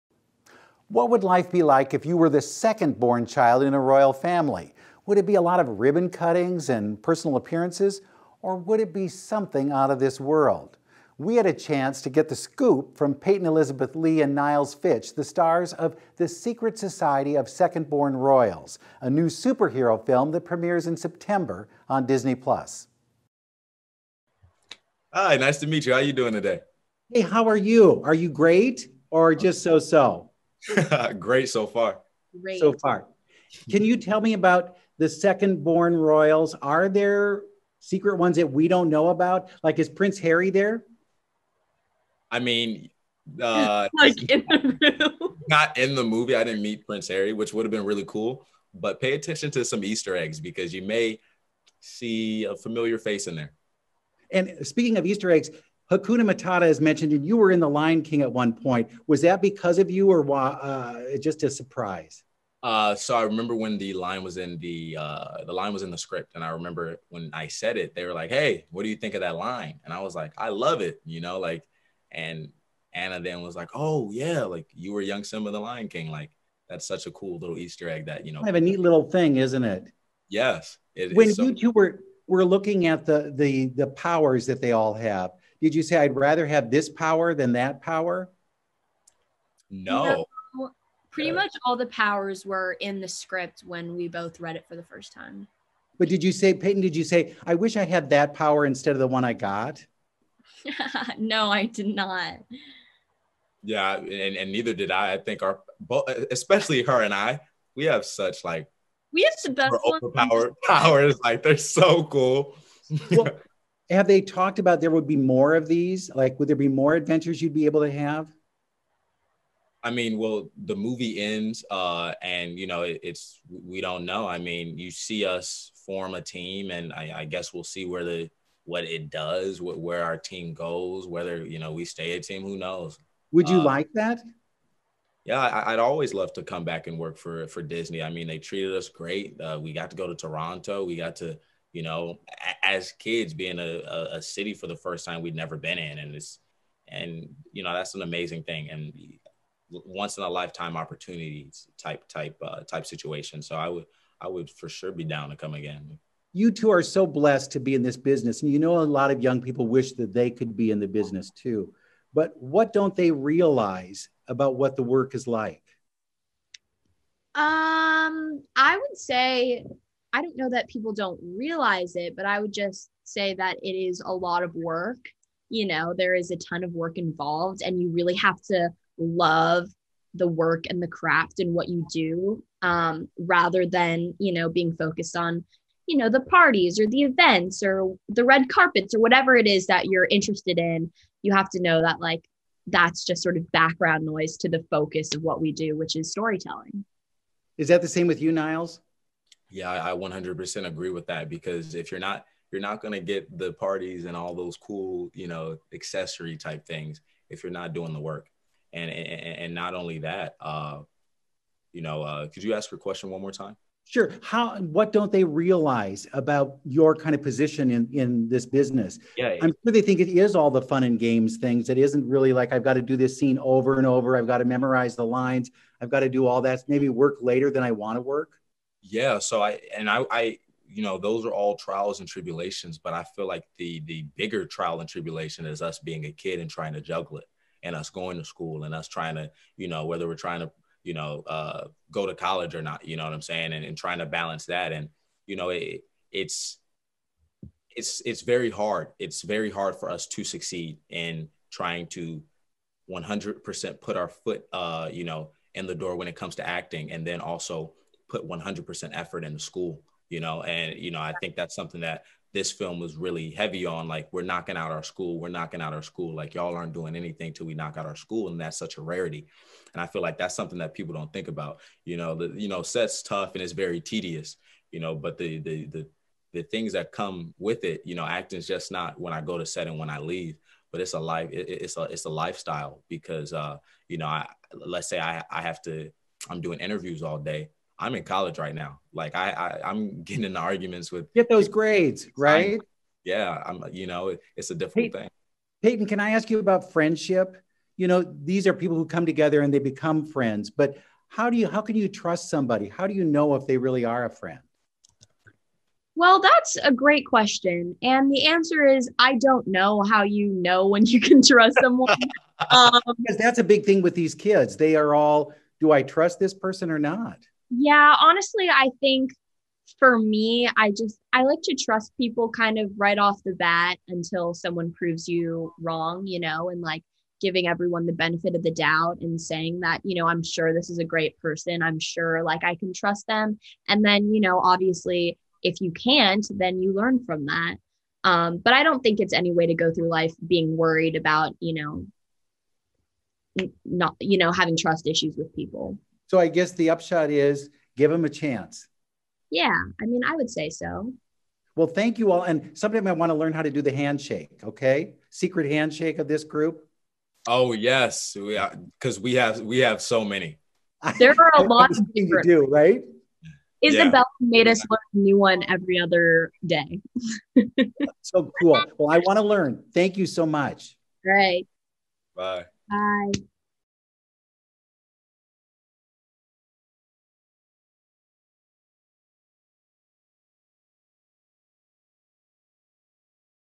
Watch Now: Meet the Stars
What don't fans understand about the life of an actor? Bruce Miller finds out from Niles Fitch and Peyton Elizabeth Lee in an exclusive video.